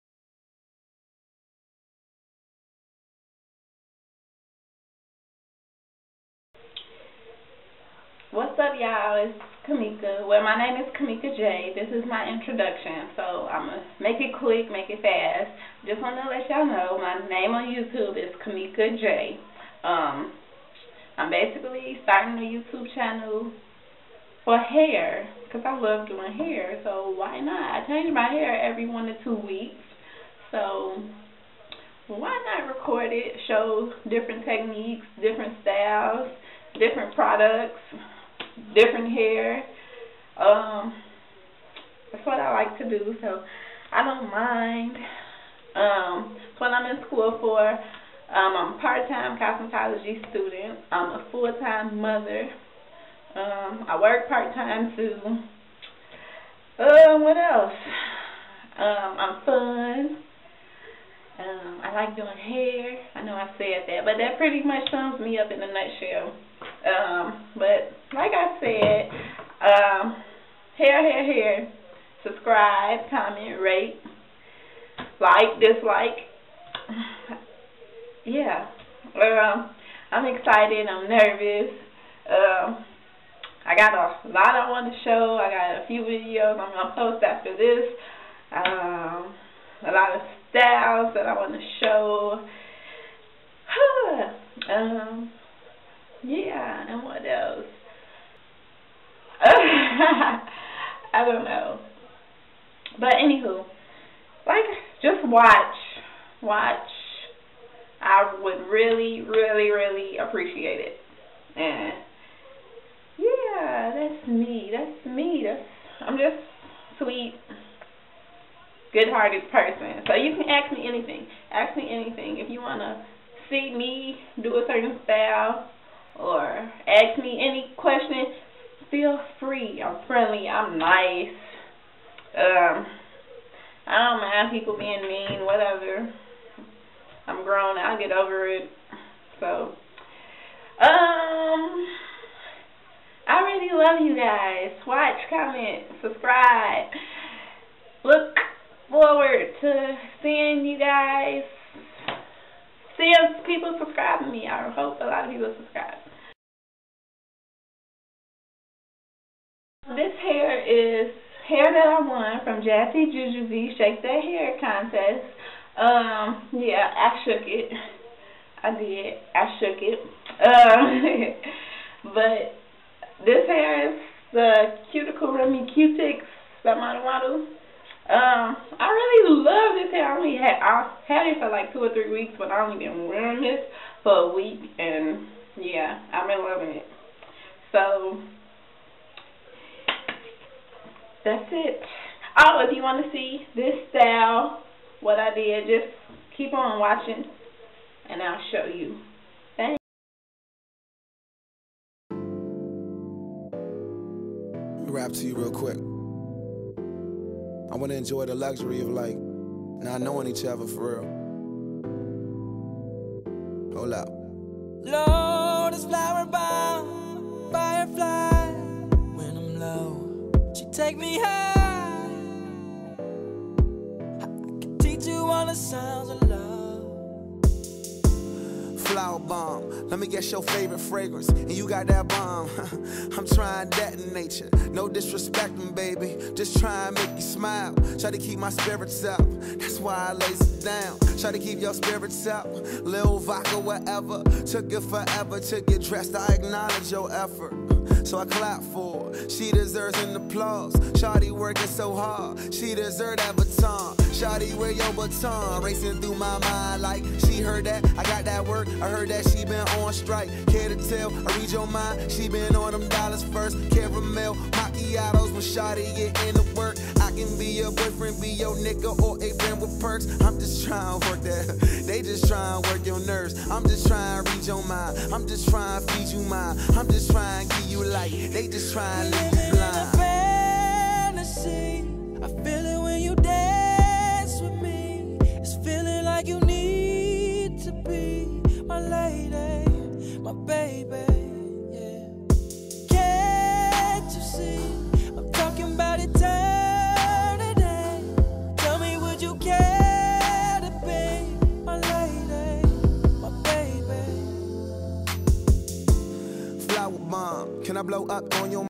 My Intro/ Chinese Bang Wig Made W/ Qutix Model Model Hair
READ ME! OMG I DONT KNOW HOW I MISPELLED PERIMETER IN THIS VIDEO... LOL IM NOT DUMB I JUST WAS TIRED WHEN I EDITED THIS. EXCUSE MY MISPELLINGS PLZ!

This video is my intro!!! I used Qutix Cuticle Remy hair by Model Model. I won this hair from JazzyJuJuBee82's Shake Your Hair Contest! Thanks!